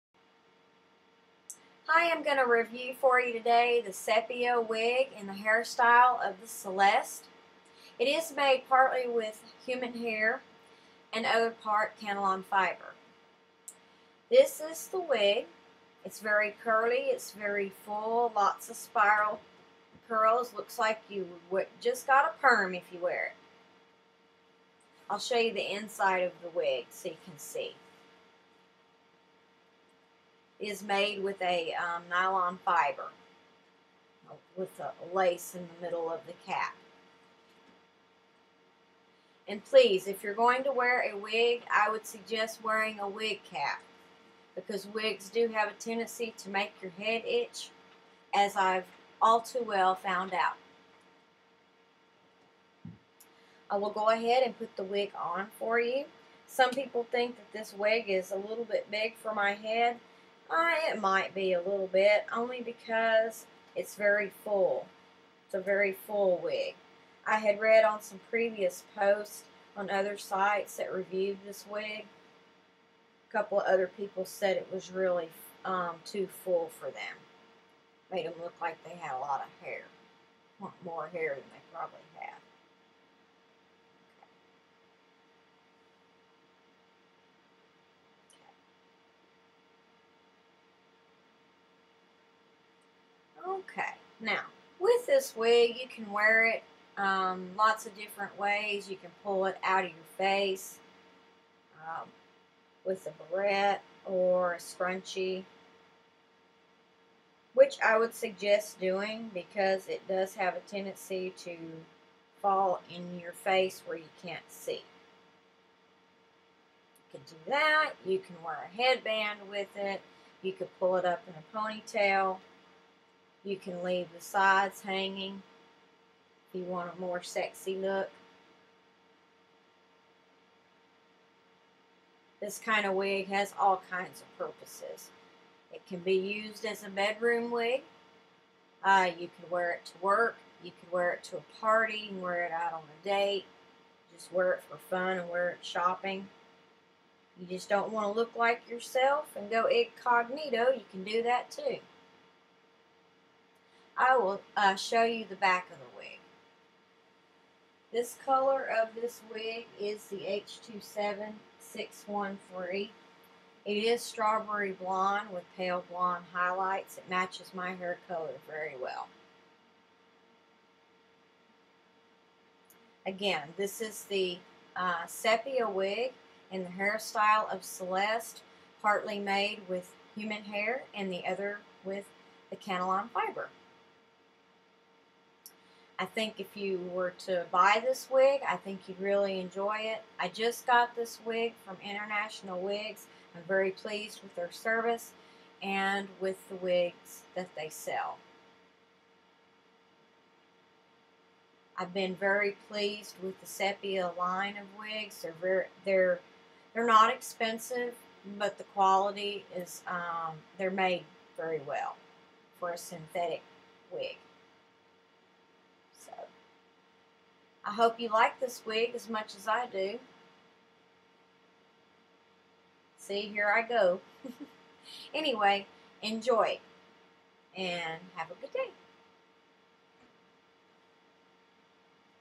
Internationalwigs.com Celeste by Sepia Wig Review
Video review of our Celeste Human Hair Blend Wig by Sepia in color MF27/613

Thank you, Natalie, for your wig video review!

Send us your wig review video and receive store credit to our website!